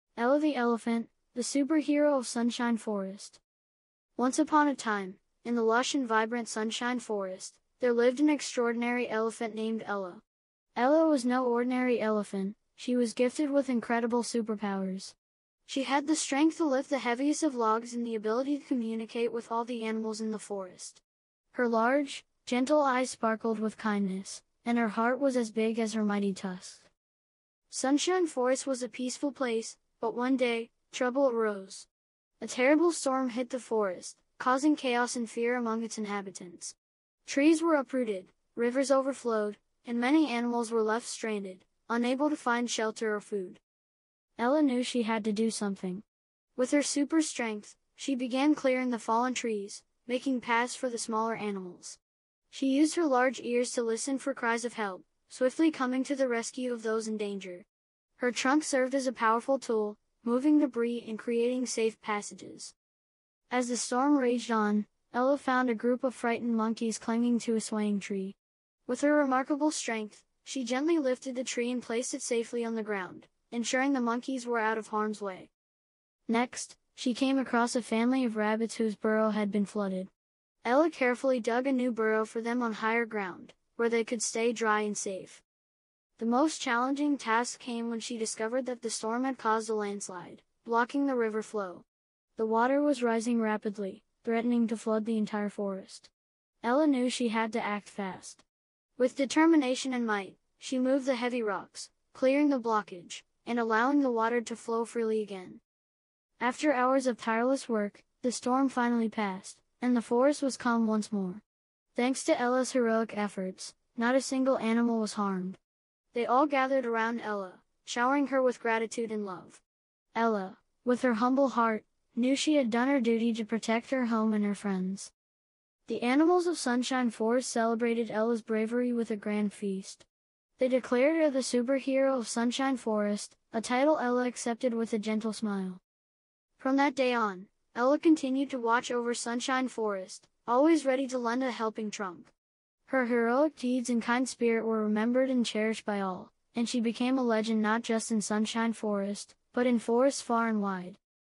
Ella the Elephant: Hero of the Forest 🐘✨- A Magical Superhero Adventure for Kids! 🌳💫
Dive into a world of adventure with "Ella the Elephant: Hero of the Forest"! 🌟 Join Ella, a superhero elephant, as she uses her incredible powers to save Sunshine Forest from a terrifying storm. This enchanting story, filled with bravery and kindness, is perfect for children who love animals and heroic tales. 🐘🌈

🔸 Why Watch?

Witness Ella's extraordinary superpowers in action!
Learn about the importance of kindness, bravery, and teamwork.
Enjoy vibrant and captivating illustrations that bring the story to life.
Ideal for kids aged 3-8, but enjoyable for the whole family.
A heartwarming tale that inspires and entertains.